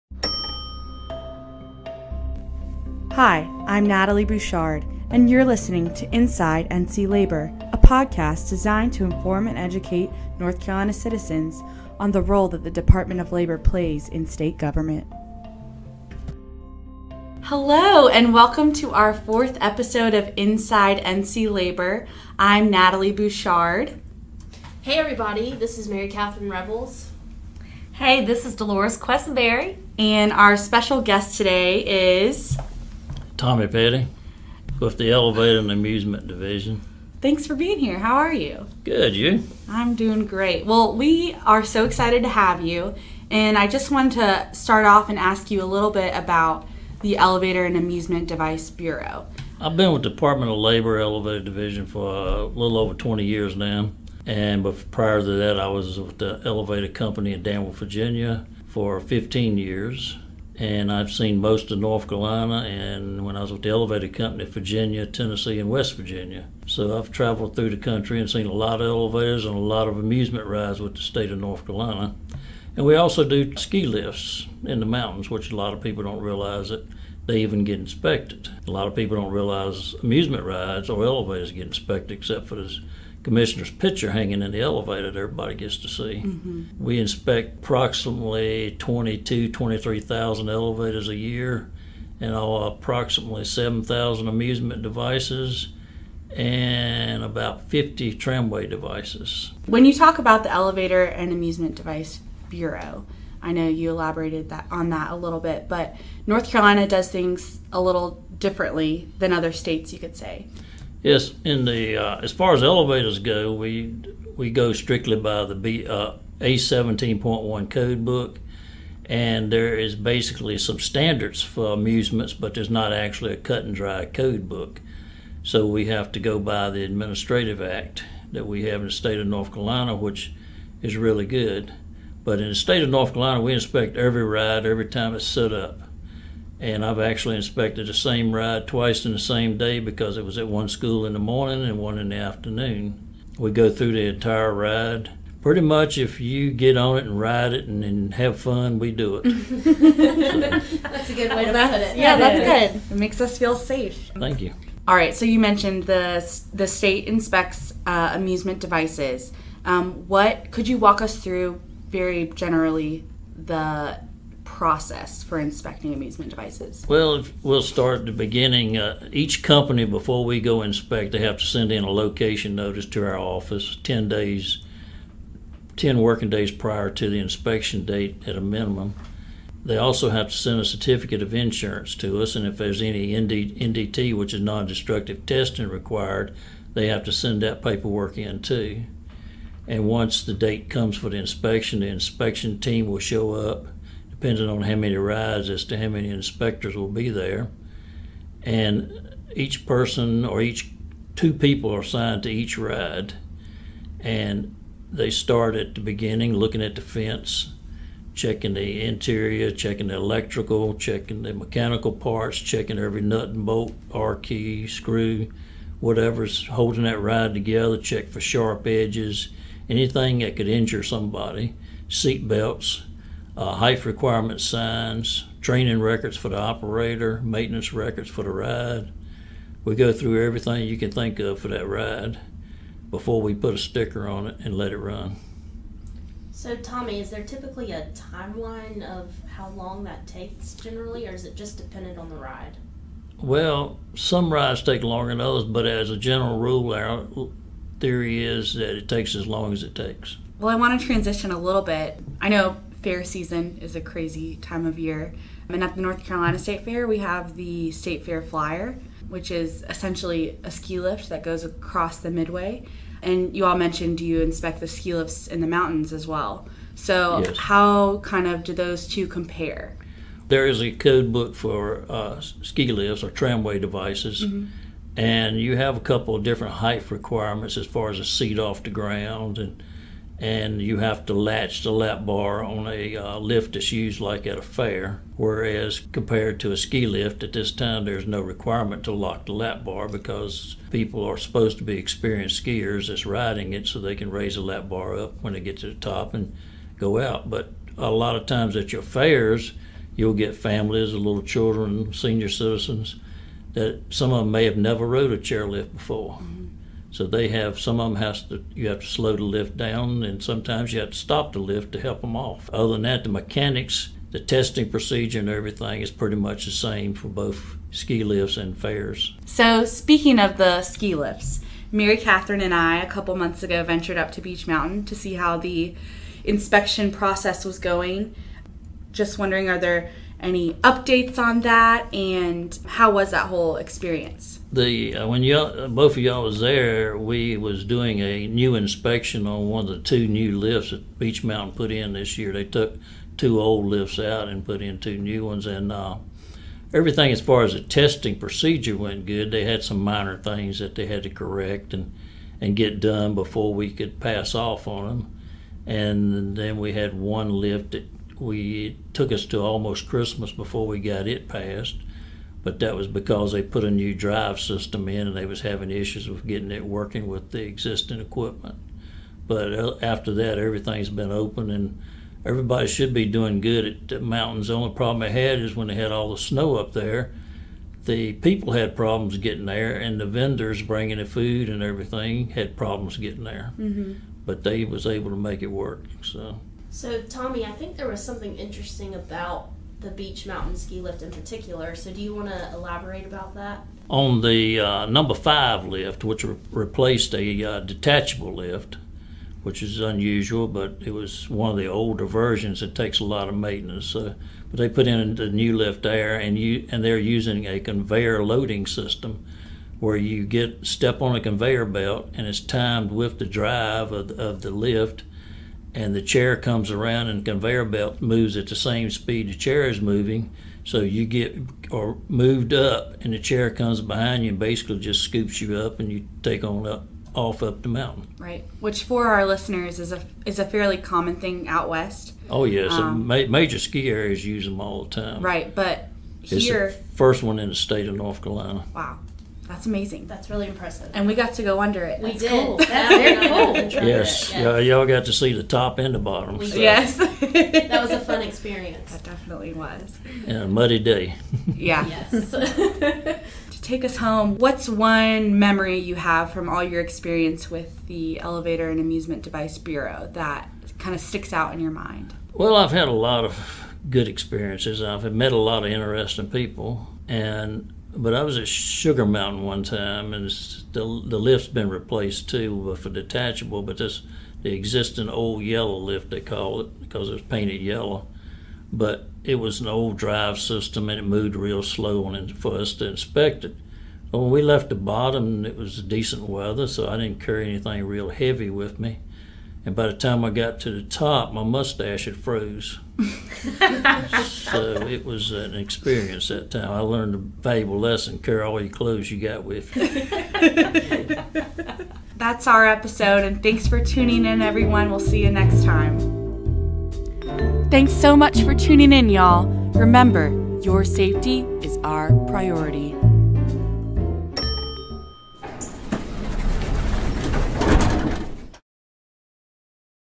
Inside NC Labor Episode 4
Episode 4 features special guest, Tommy Petty, Elevator and Amusement Device Bureau.